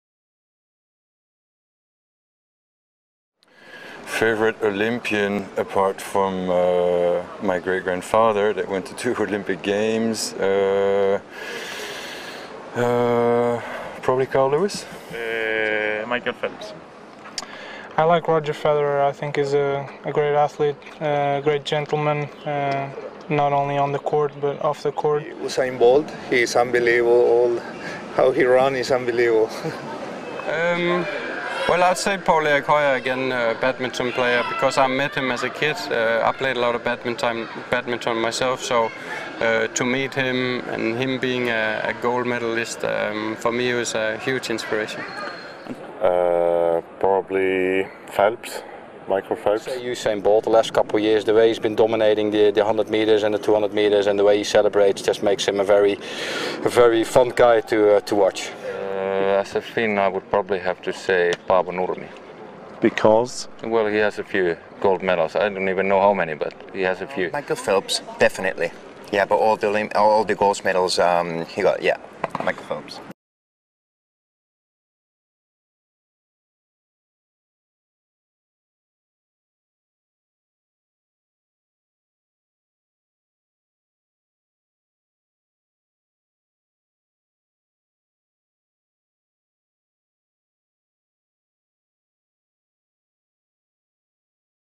Ask Olympic golfers: 'who are your favourite Olympians of all time?'
For mor info visit:
po.st/Rio2016Golf
po.st/OlympicGolfRio